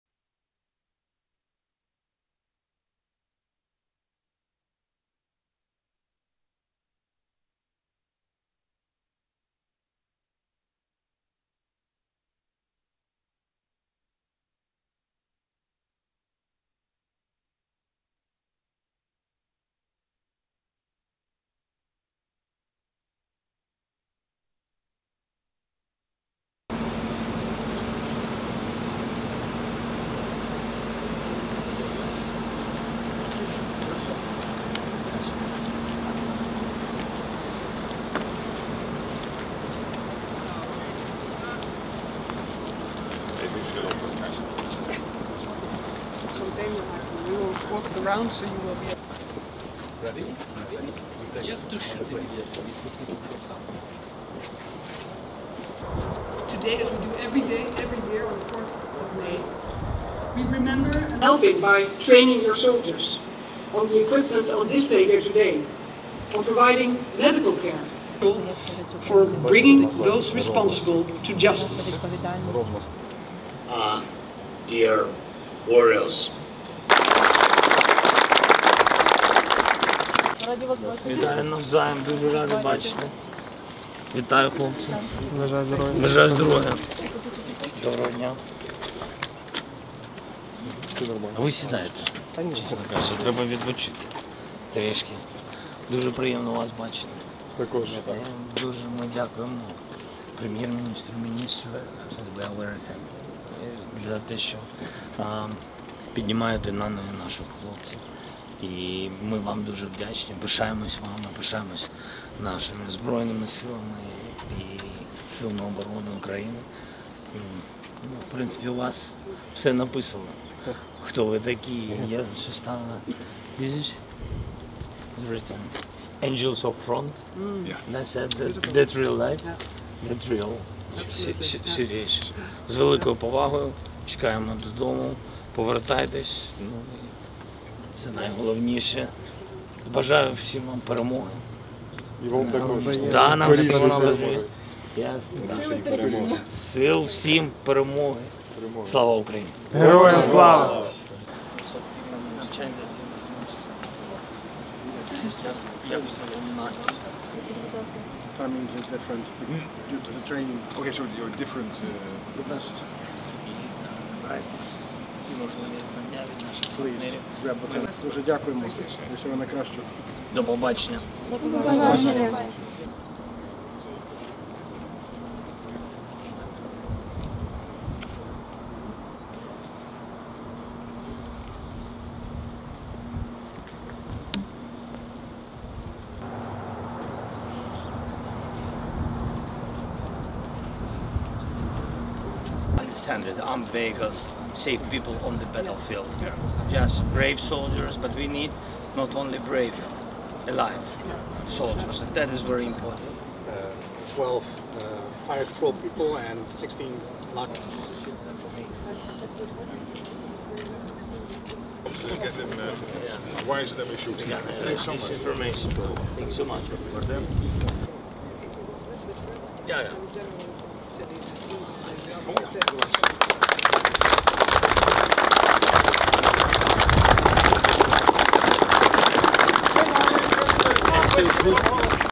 У Нідерландах Зеленському продемонстрували військову техніку, яку буде відправлено в Україну
Тут і зараз ми бачимо, що таке допомога та що таке захист життя. Це найпочесніша місія – робити все, щоб життя перемогло.

Зброя із цієї бази, яка допомагає нашим захисникам, берегтиме життя – нормальне, вільне, ґрунтоване на повазі життя українців та всіх європейців.

Слава всім, хто допомагає!

Here and now, we see what help is and what life protection is. This is the most honorable mission – to do everything possible to ensure that life prevails.

The weapons from this base that help our defenders will save life – normal, free life of Ukrainians and all Europeans based on respect.

Glory to everyone who helps!